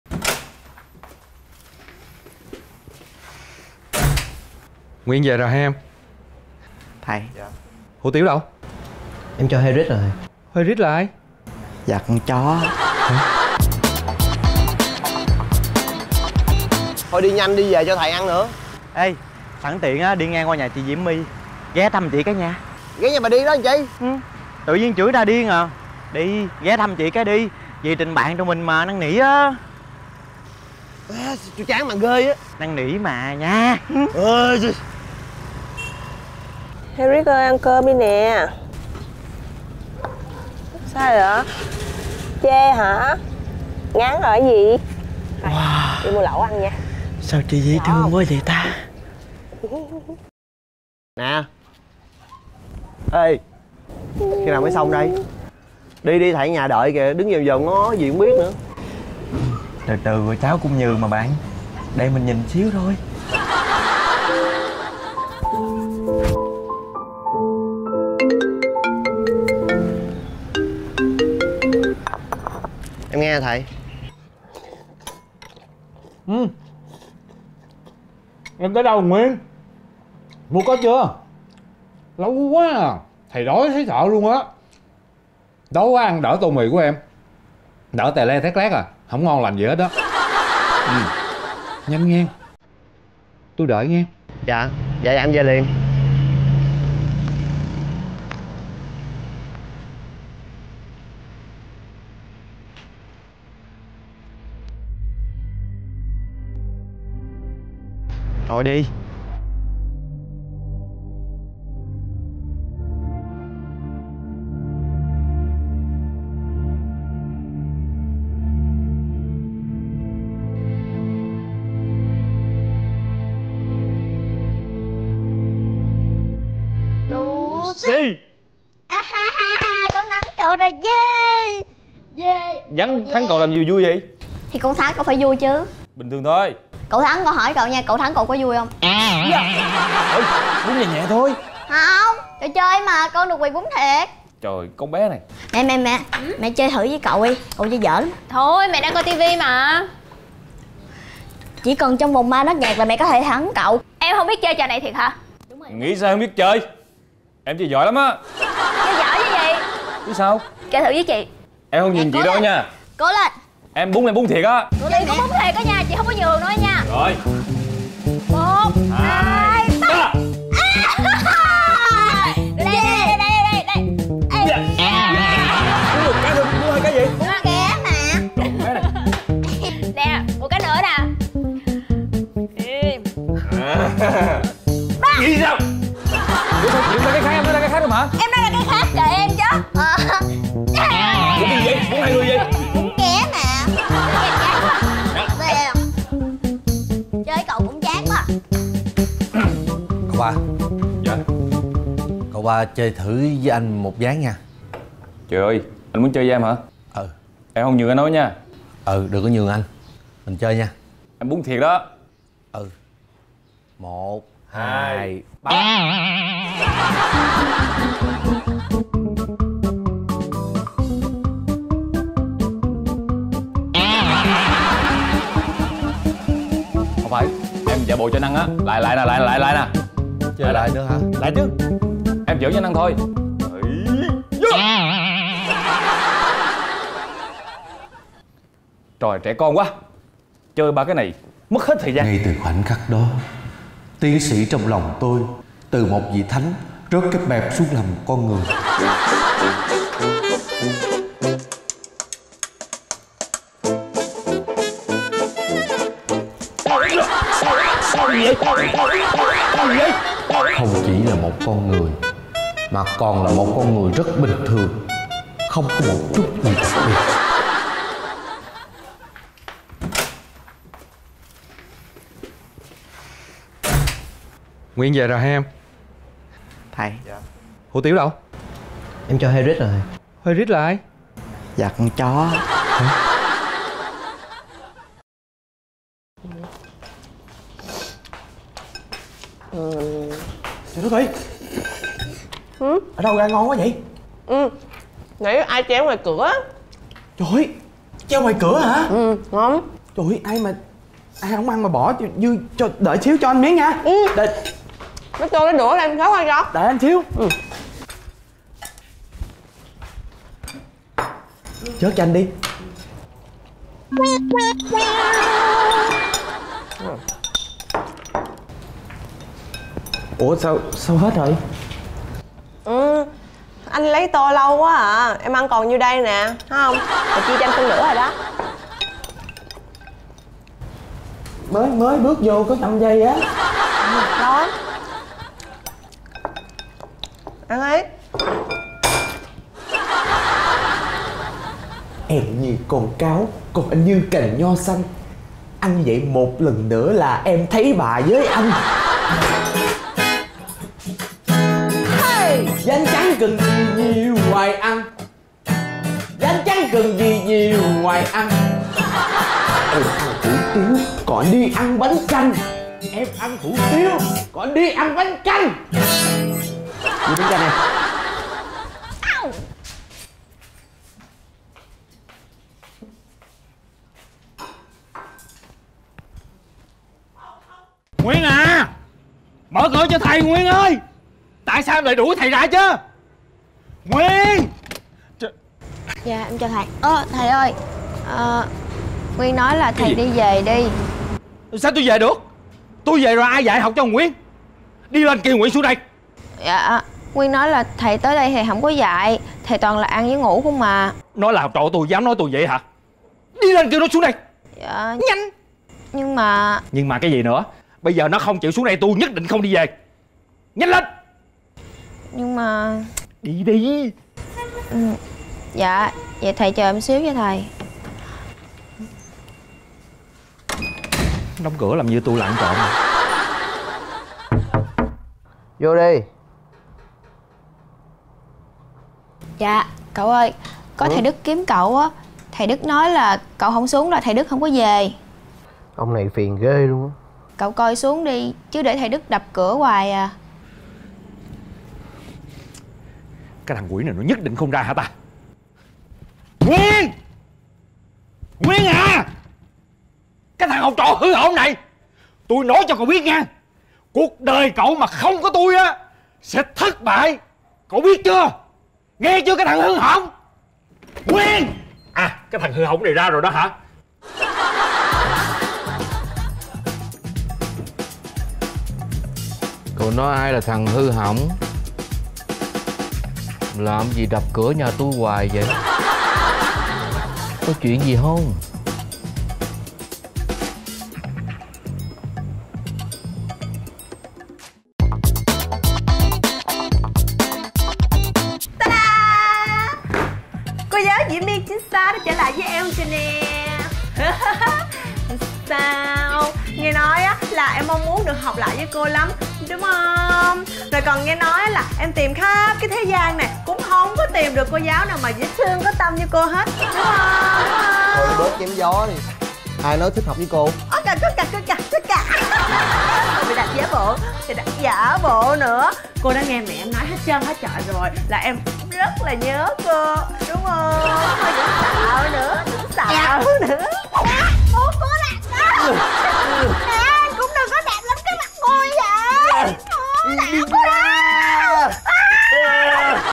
Thầy giáo "tào lao" toàn ăn với ngủ | Fast TV
Đóng tiền học phí mà thầy như thế này thì chẳng ra gì.